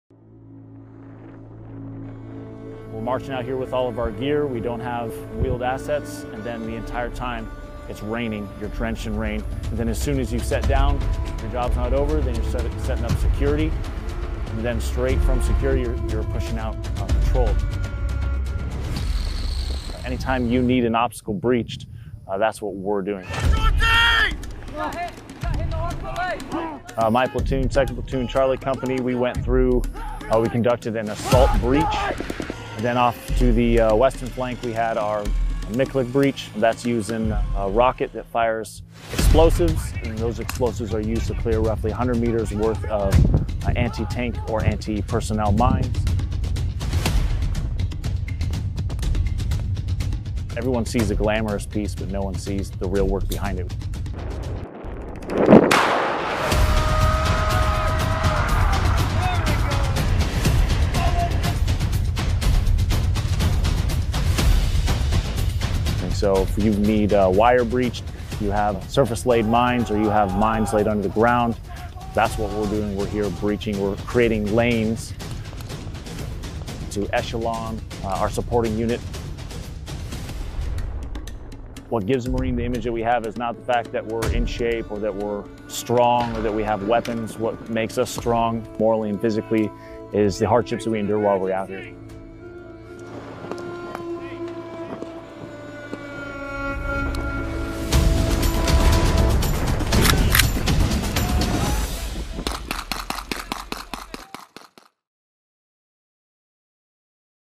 US Military News • U.S. Marine Combat Engineers • Combat Readiness Evaluation • June 7 2021
U.S. Marines with 2d Combat Engineer Battalion, 2d Marine Division, participate in a demolition range as part of a Marine Corps Combat Readiness Evaluation on Camp Lejeune, N.C., June 7, 2021.

The purpose of the range was to build Military core proficiency, improve combat readiness and to better prepare for combat scenarios before deployment.

"The appearance of US Department of Defense (DoD) visual information does not imply or constitute DoD endorsement."

Credit to : U.S. Marine Corps video by Lance Cpl. Skylar M. Harris